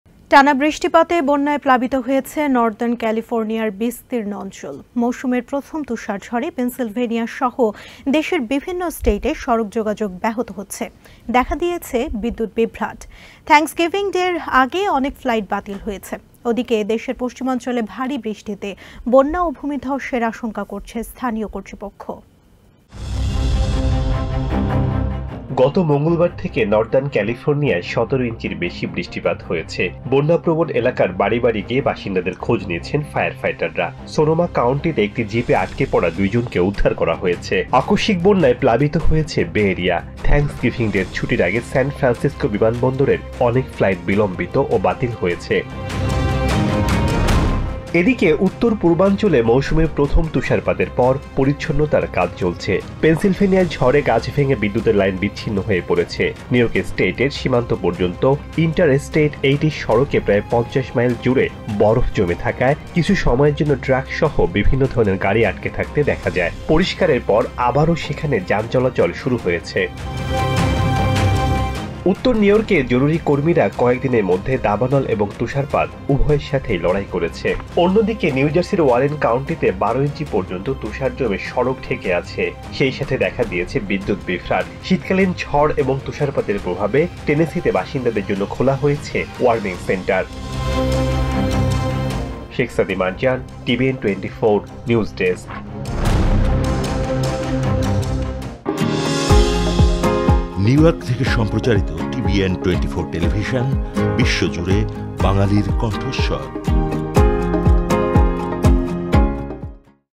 বন্যা ও তুষারঝড়ে বিপর্যস্ত অ্যামেরিকার বিভিন্ন স্টেইট | TBN24 NEWS | Northern California | Flood
টানা বৃষ্টিপাতে বন্যায় প্লাবিত হয়েছে নর্দার্ন ক্যালিফোর্নিয়ার বিস্তীর্ণ অঞ্চল। মৌসুমের প্রথম তুষার ঝড়ে পেনসিলভেনিয়াসহ দেশের বিভিন্ন স্টেইটে সড়ক যোগাযোগ ব্যাহত হচ্ছে। দেখা দিয়েছে বিদ্যুৎ বিভ্রাট।

বন্যা ও তুষারঝড়ে বিপর্যস্ত অ্যামেরিকার বিভিন্ন স্টেইট | TBN24 NEWS | Northern California | Flood | Northeast | Major Snowfall | Weather Breaking News